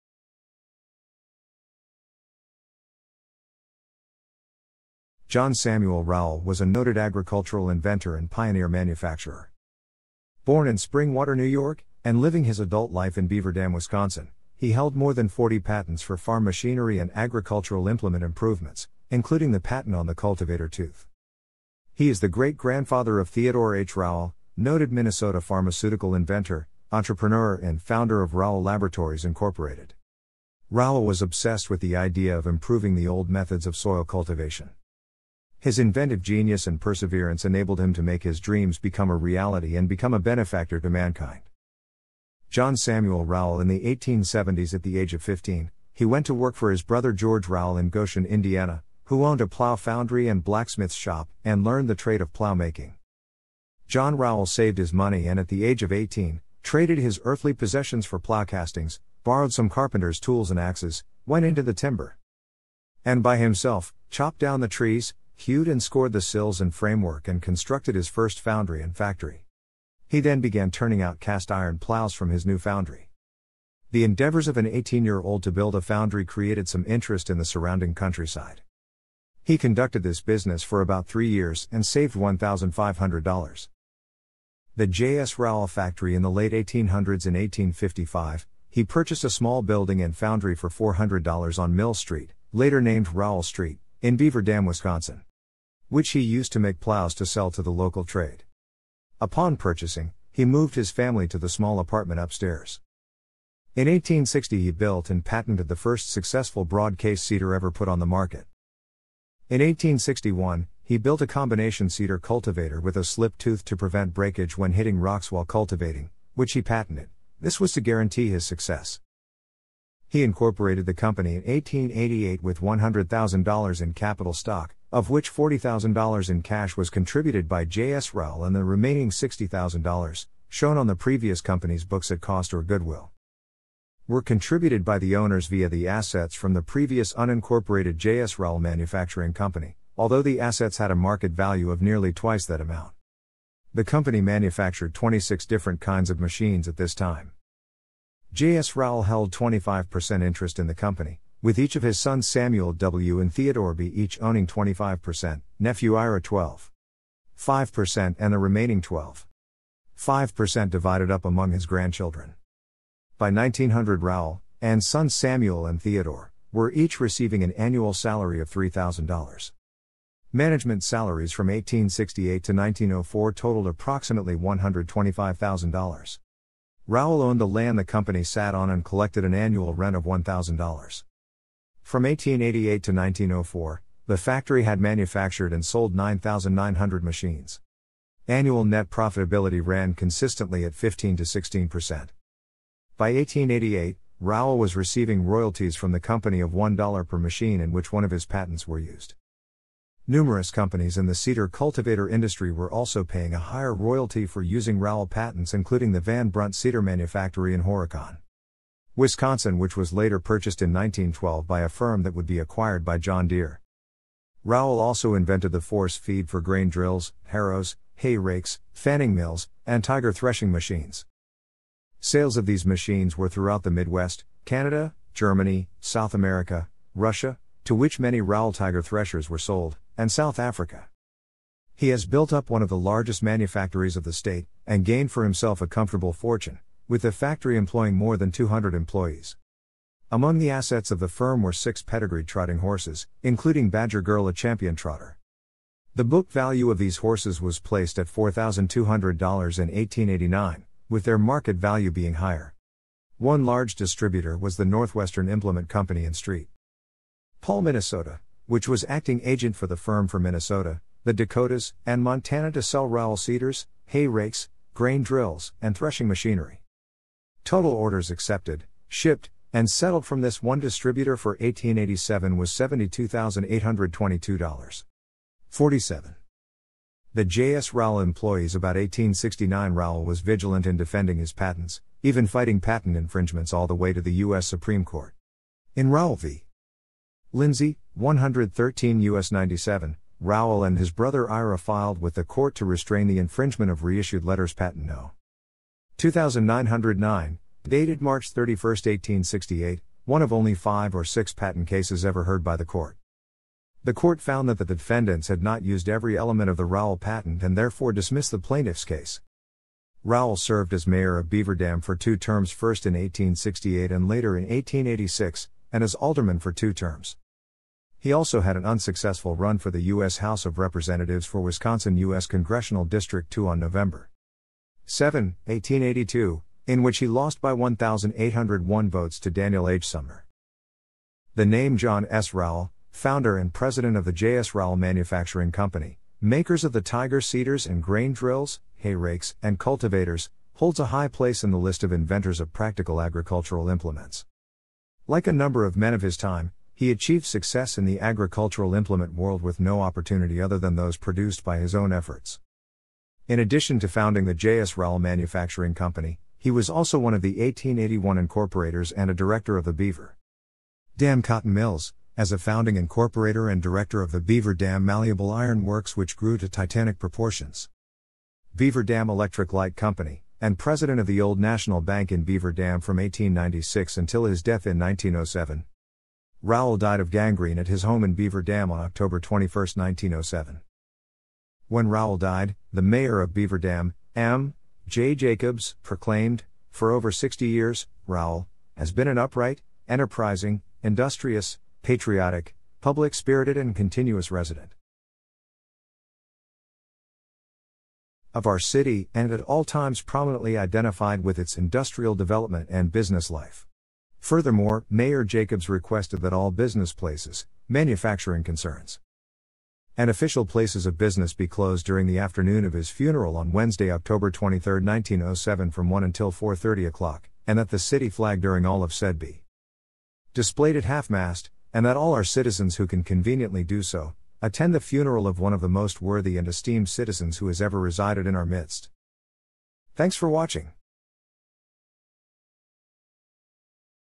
John Samuel Rowell
19th-century_American_inventors
People_from_Livingston_County,_New_York
People_from_Beaver_Dam,_Wisconsin
19th-century_American_politicians
John Samuel Rowell （April 1, 1825 – October 21, 1907） was a noted agricultural inventor and pioneer manufacturer.
Born in Springwater, New York, and living his adult life in Beaver Dam, Wisconsin, he held more than 40 patents for farm machinery and agricultural implement improvements, including the patent on the cultivator tooth.
He is the great grandfather of Theodore H. Rowell, noted Minnesota pharmaceutical inventor, entrepreneur, and founder of Rowell Laboratories, Inc.
Rowell was obsessed with the idea of improving the old methods of soil cultivation.
His inventive genius and perseverance enabled him to make his dreams become a reality and become a benefactor to mankind.
John Samuel Rowell in the 1870s At the age of 15, he went to work for his brother George Rowell in Goshen, Indiana, who owned a plow foundry and blacksmith's shop, and learned the trade of plow making.
John Rowell saved his money and at the age of 18, traded his earthly possessions for plow castings, borrowed some carpenters tools and axes, went into the timber,
and by himself, chopped down the trees, hewed and scored the sills and framework and constructed his first foundry and factory.
He then began turning out cast iron plows from his new foundry.
The endeavors of an 18‑year‑old to build a foundry created some interest in the surrounding countryside.
He conducted this business for about 3 years and saved $1,500.
The JS Rowell factory in the late 1800s In 1855, he purchased a small building and foundry for $400 on Mill Street, later named Rowell Street, in Beaver Dam, Wisc.